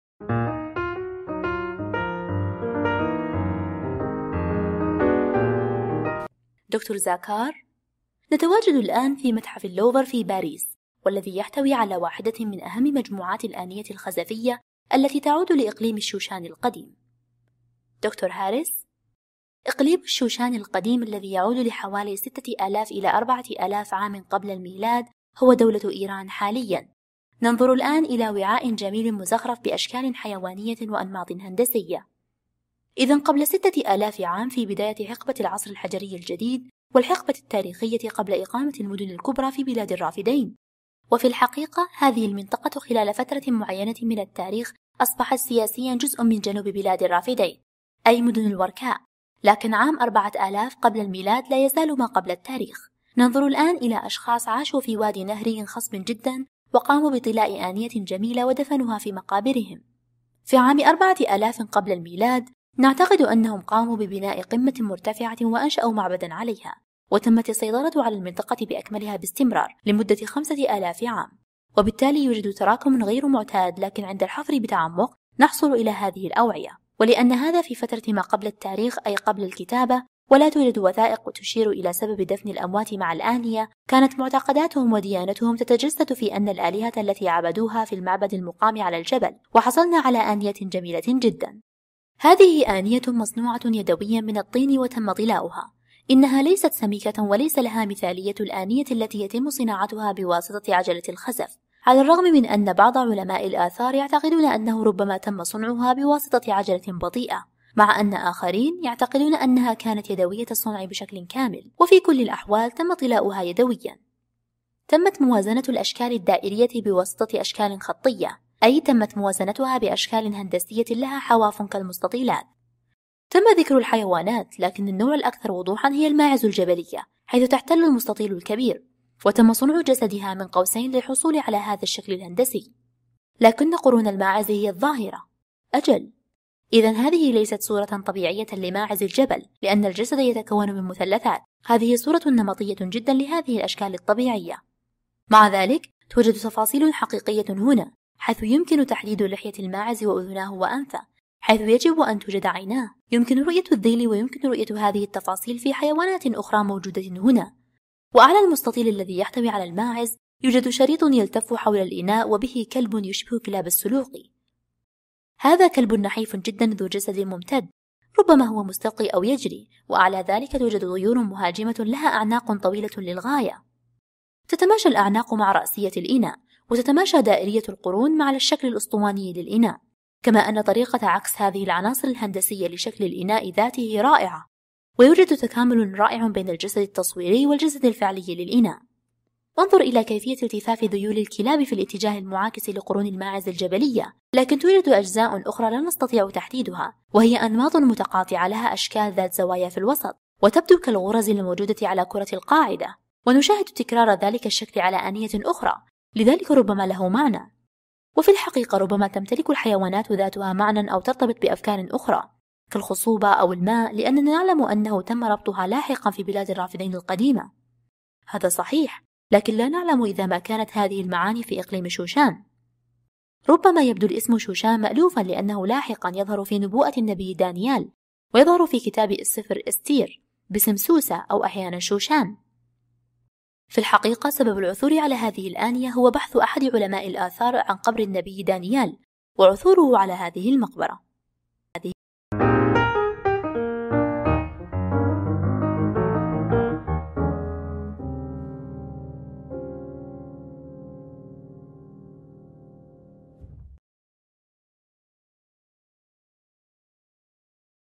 مكيال الحبوب ذو زخارف حيوان الوَعْل
مكيال الحبوب ذو زخارف حيوان الوَعْل يعود للفترة بين عام أربعة آلاف ومائتين وعام ثلاثة آلاف وخمسمائة قبل الميلاد في إقليم شوشان.
وصف الفيديو:
مكيال الحبوب ذو زخارف حيوان الوعل يعود للفترة بين عام أربعة آلاف ومائتين وعام ثلاثة آلاف وخمسمائة قبل الميلاد تعودلحقبة شوشان، المدينة الجنائزية، جيل أَكْرُوبُولْس، إقليم شُوشَان، إيران. هي عبارة عن طين نضيج مطلي بأبعاد (ثمانية وعشرين سنتيمتر وتسعين مليمتر في ستة عشر سنتيمتر وأربعين مليمتر). تم العثور عليه خلال حفريات قادها "جَاكْ دِي مُورغَان" بين عام ألف وتسعمائة وستة وعام ألف وتسعمائة وثمانية، ويوجد الآن في متحف اللوفر.